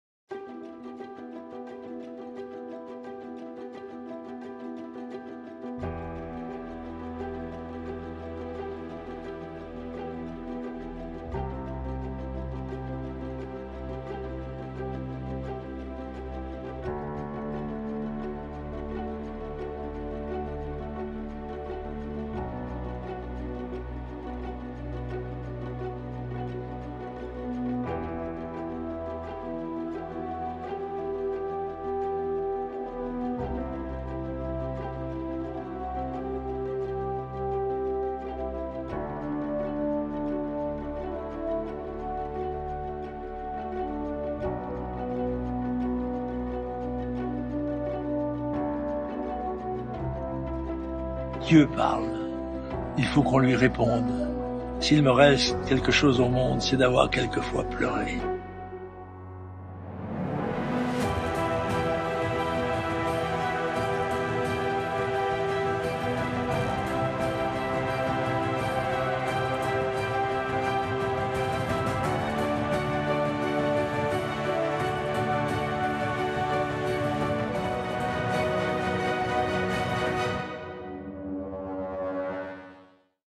Rencontre avec Jean-Marie Le Pen
La Nouvelle Librairie vous donne rendez-vous au 11, rue Médicis, le vendredi 4 octobre de 17h à 20h, pour une rencontre exceptionnelle avec Jean-Marie Le Pen, à l'occasion de la sortie du tome II de ses Mémoires.